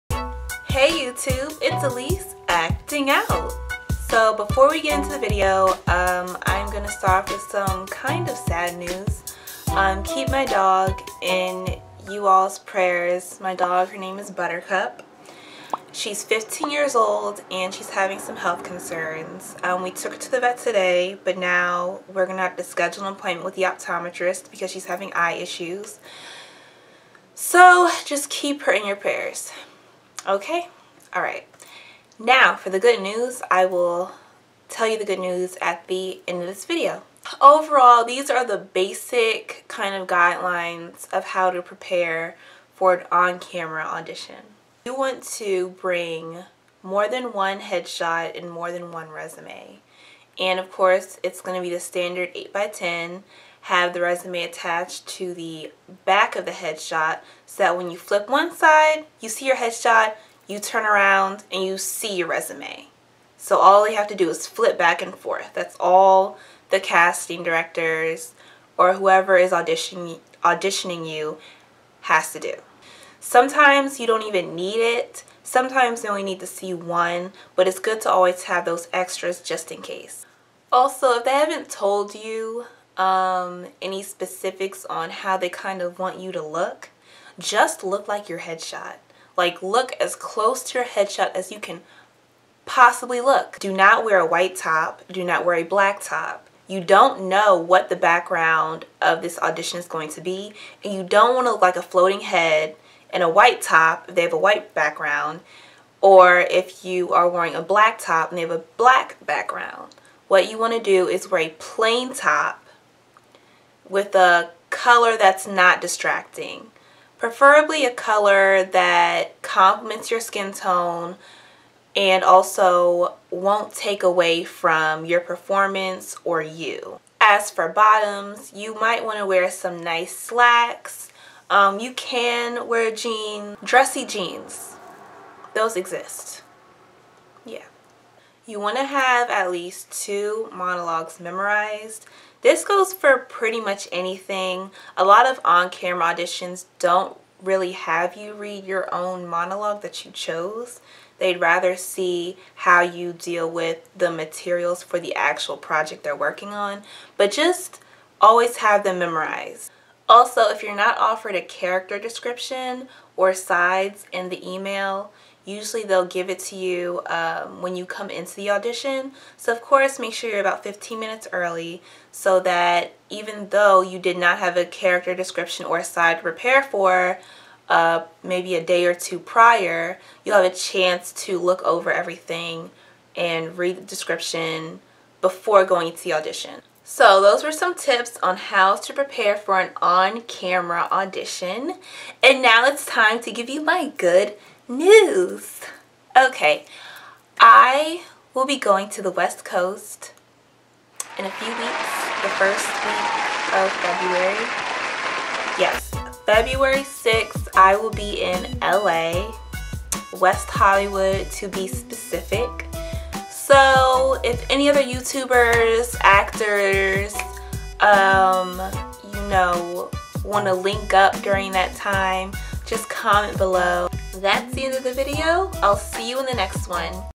How To: Prepare for an On Camera Audition
Hey guys! Here is another how to acting video to any beginners interested in breaking into the acting industry. These are just basics of what you will need for every audition.

SOCIAL MEDIA
Facebook: Alyse - Acting Out